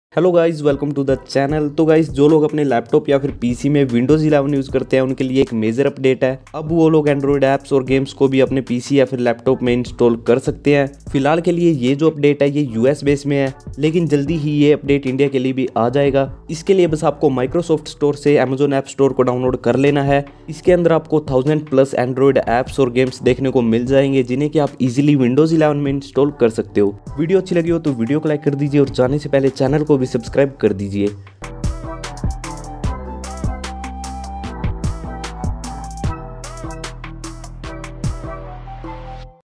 WINDOWS 11 NEW UPDATE 2022 :- SUPPORT ANDROID APPS AND GAMES | windows subsystem for android
Windows 11 New update in 2022 Android app support & New features out now. Windows 11 was released last year with that android app support also released later about in 3 4month but finally after a long wait here is the final update for windows 11 users now all windows versions are able to install android games on it without any problem currently it roll out in the US but mostly arrive in India in  few weeks

The new update brings all features like complete android support with that changes in the taskbar and new apps interface for a notepad and other application